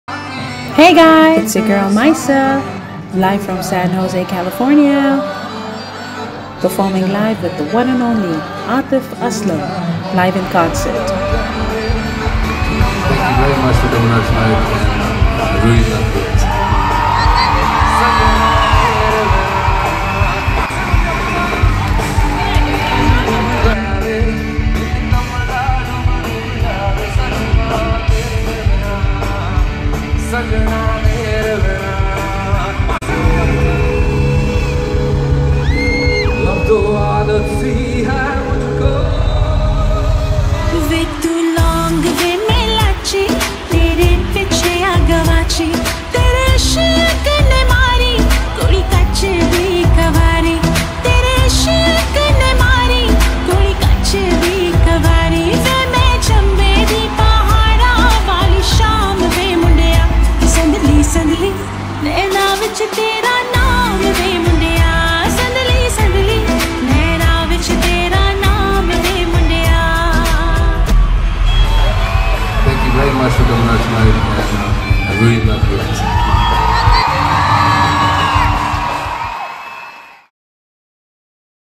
Atif Aslam & Myssah Live in Concert - San Jose California
♫ Pakistani Playback Singer Atif Aslam & Canadian Music Sensation Myssah  ♫
Live in Concert  Video  at San Jose California